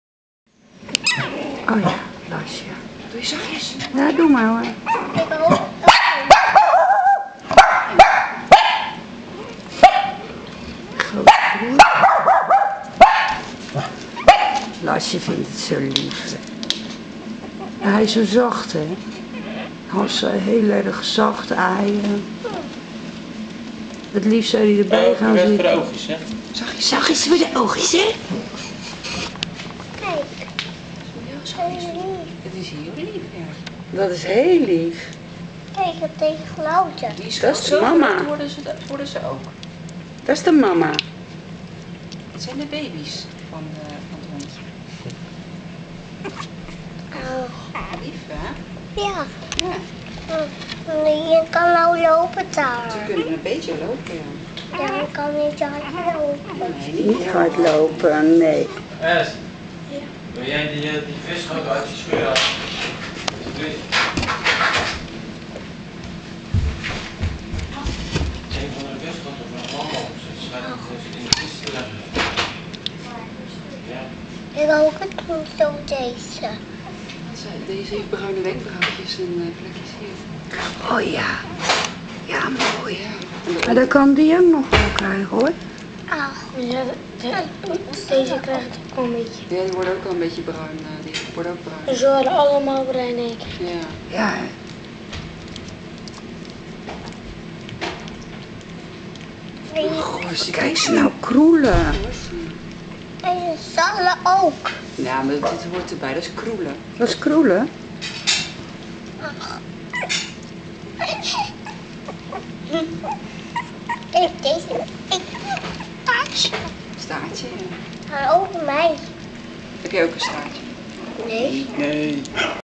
Lhasa Apso Pupjes 3.5 week op 13 sept. 2011.WMV
Ons Lhasa pupje met haar 2 zusjes en broertje.
Ze zijn geboren op 20 augustus 2011.